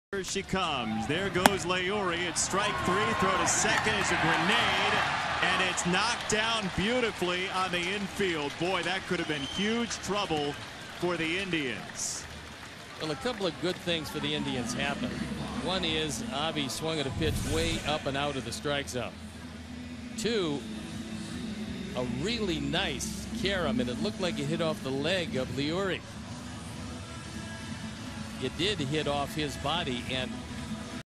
CLE@CWS: L. Garcia swipes second base in a big spot
Leury Garcia gets a great jump and successfully steals second base in the bottom of the 9th inning with the game tied at 1